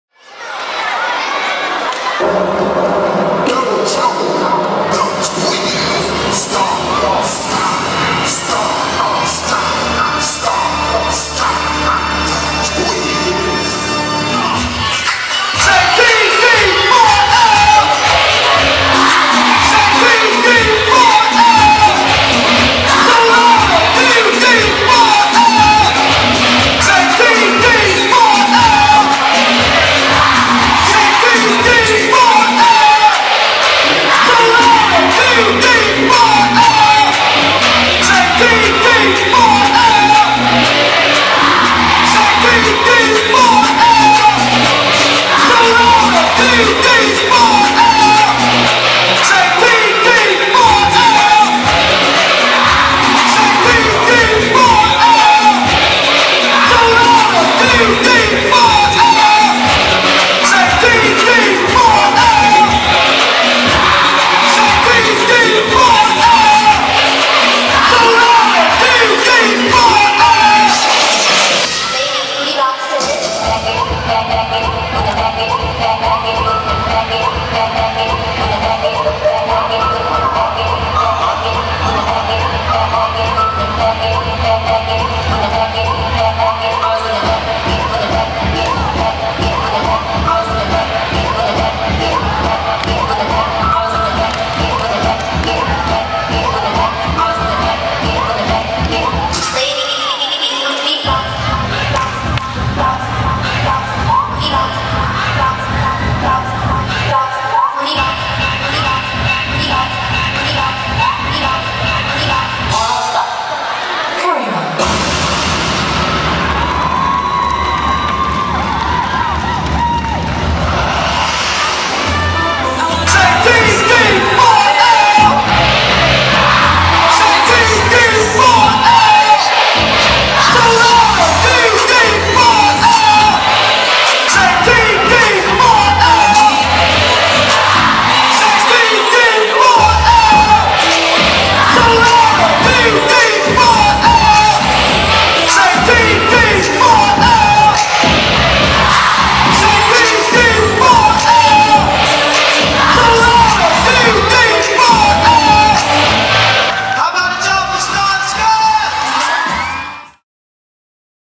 Star and Sky Shine Bright in Holly Springs Dance Spectacular! ✨💃
Star , Sky from DD4L perform in Holly Springs Mississippi
Prepare to be dazzled as the dynamic duo, Star and Sky, from the legendary Dancing Dolls, set the stage on fire in Holly Springs, Mississippi! 🔥🌟 With their exceptional talent, synchronized moves, and infectious energy, Star and Sky are ready to deliver a performance that will leave you breathless. Don't miss this electrifying showcase of dance artistry and sisterly synergy! StarAndSky DancingDolls DD4L HollySprings Mississippi DanceSpectacular UnforgettablePerformance"